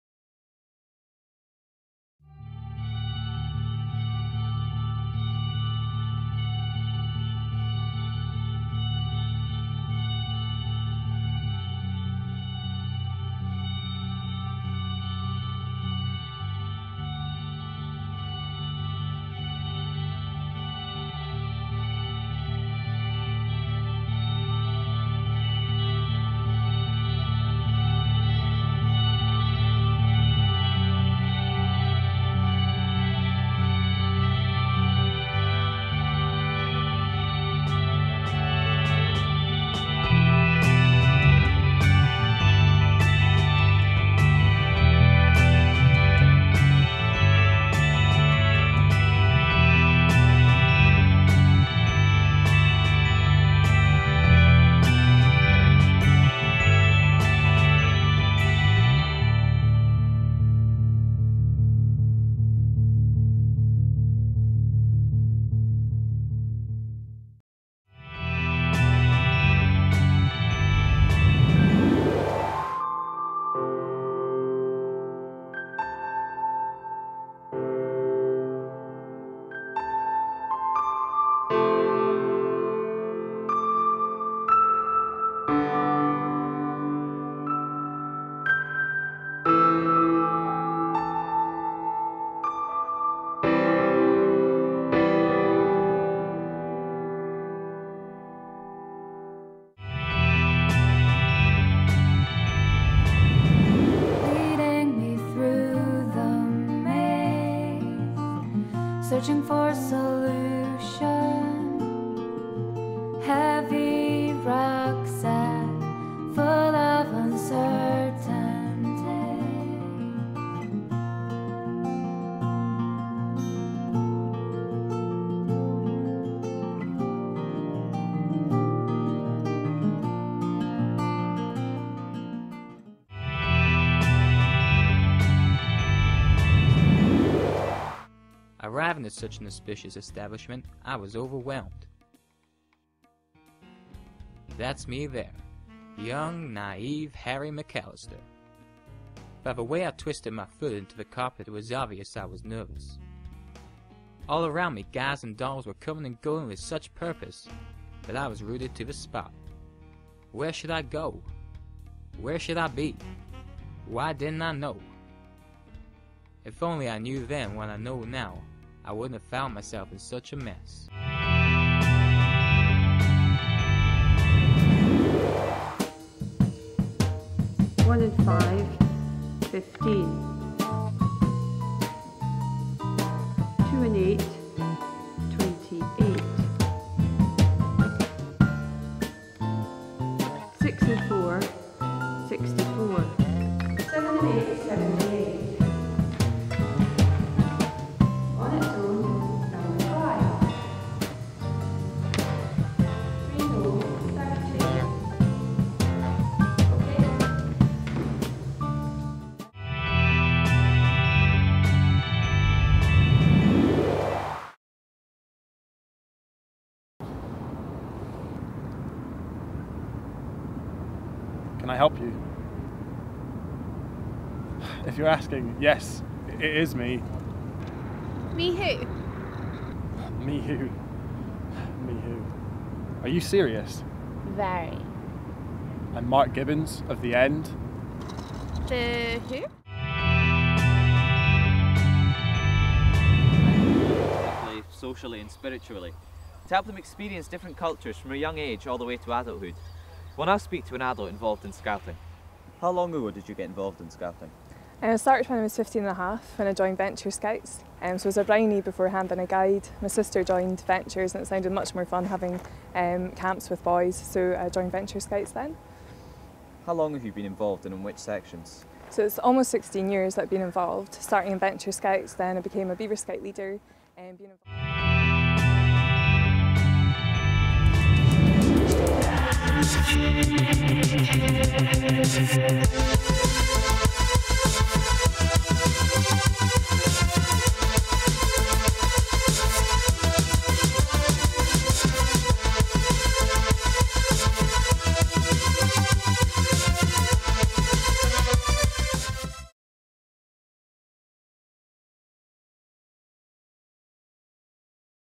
Michael Dart Showreel 2013
A showreel that compiles all of my work to date.

Productions featured are as follows:

Fallow (Director/Camera/Editor)

Ghost in the Machine (Director/Camera/Editor)

Grim (Director/Writer/Editor)

Induction Day (Director/Editor)

Scouting in the 21st Century (Camera Operator)

Stonehaven Advert (Director/Camera/Editor)

Substitute (Director/Writer/Editor)

Created May 2013.